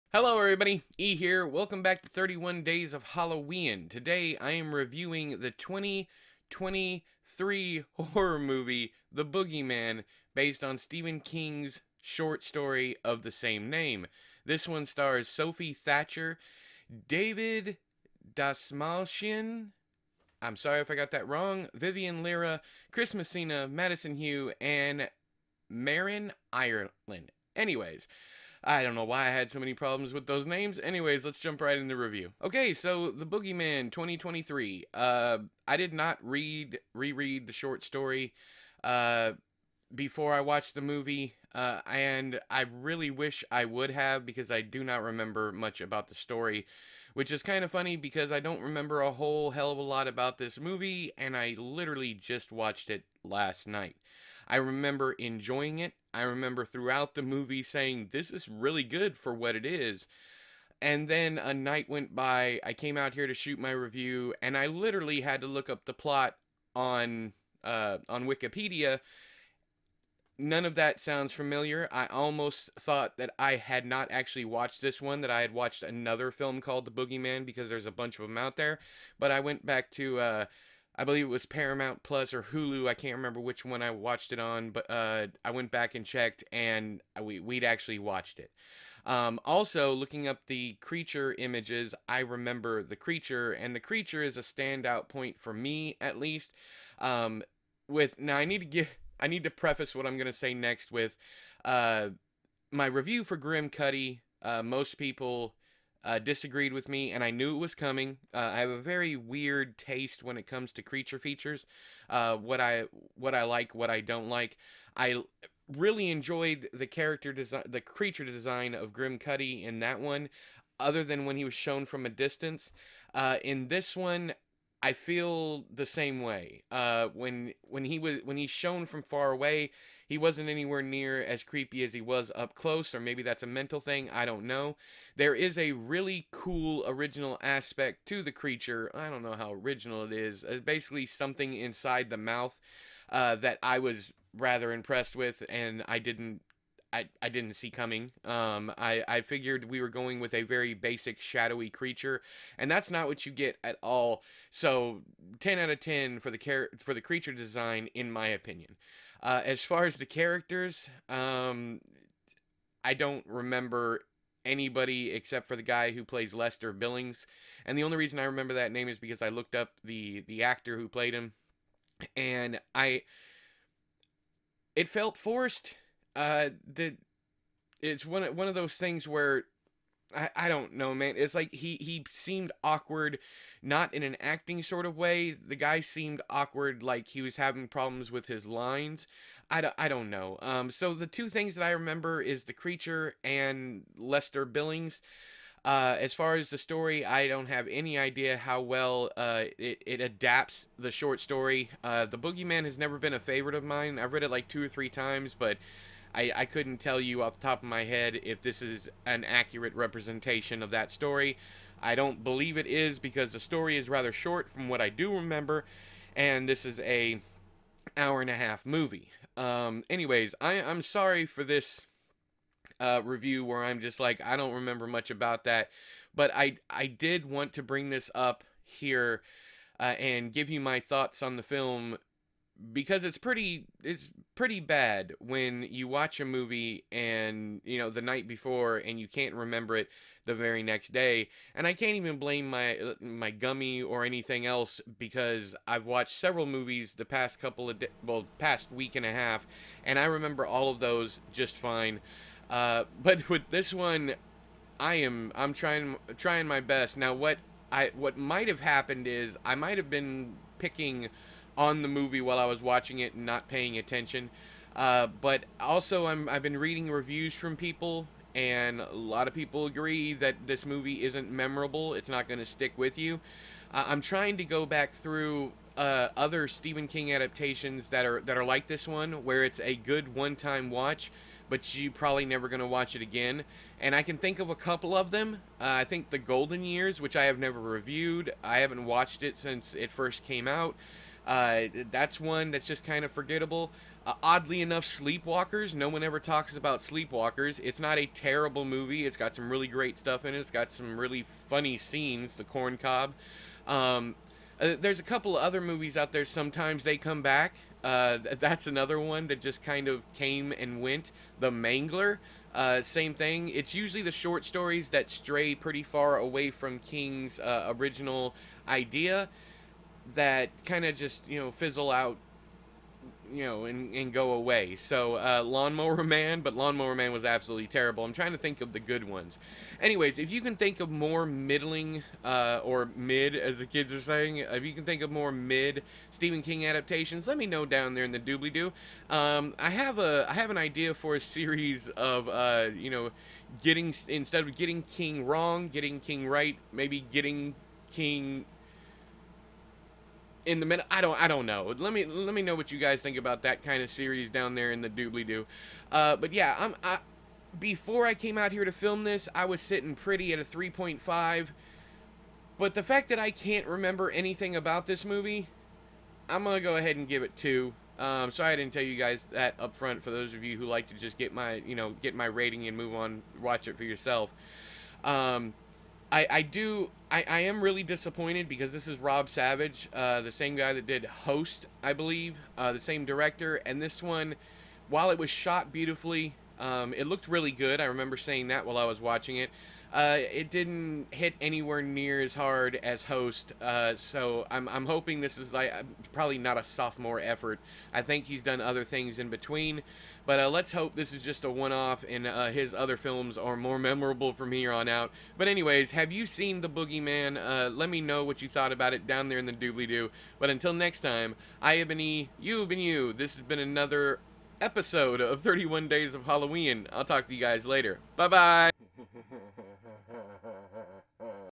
31 DAYS OF HALLOW-E'N | THE BOOGEY MAN (2023) | Movie Review
DAY 13! I'm your host E., and today I'm reviewing THE BOOGEYMAN (2023)!

Super Chats and Super Thanks are appreciated! Even simple engagement (comments, likes, and yes, even dislikes) helps immensely.